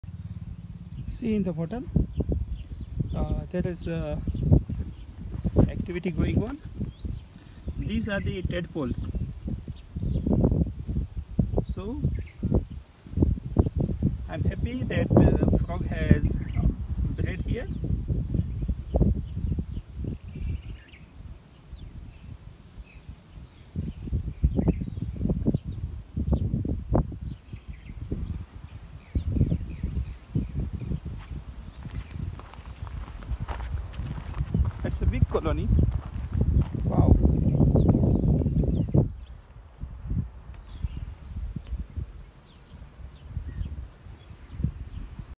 FARM frog tadeple
in my farm we have teadpoles running in freshwater. this is a good thing.